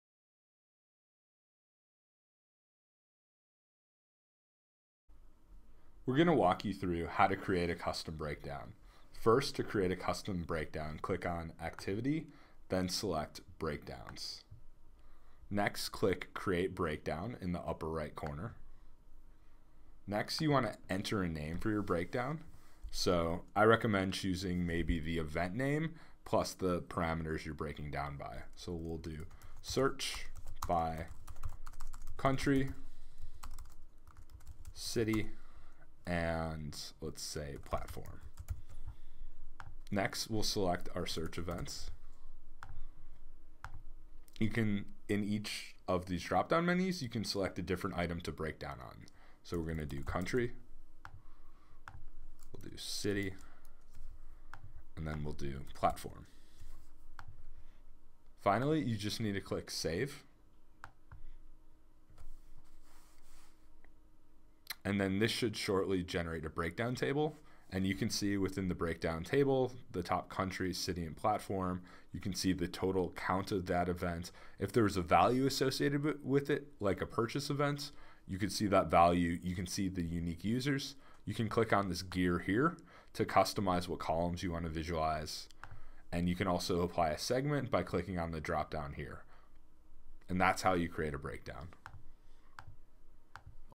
Create a Custom Breakdown
Answer questions such as, "How much of my revenue is coming from a particular platform-operating system combination in a particular country?" and learn how to create a breakdown to help you slice and dice your data.

To get started with Facebook Analytics for Apps, visit our quickstart guide